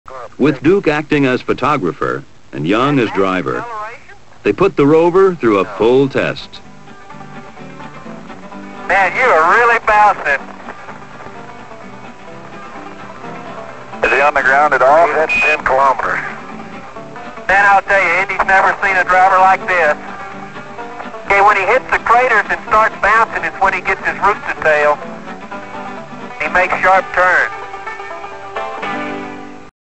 Apollo 16 - Driving on the Moon! | 51 Years Ago Today!
On this 51st anniversary of the first day of lunar exploration for Apollo 16 astronauts John Young and Charlie Duke at the Descartes Highlands, this edition of @mannedspace1187 takes a look at John Young putting the Lunar Roving Vehicle through its paces for the first time.

Footage: NASA

Footage: NASA